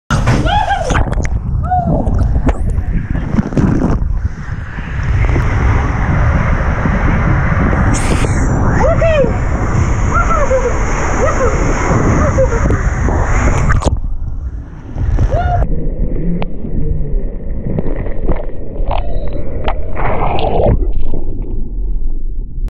Volcano Bay - Tai Nui Serpentine Body Slide [POV]
Tai Nui Serpentine Body Slide is an awesome tube drop slide. There are two tubes you can go down, and the Green one is Tai Nui.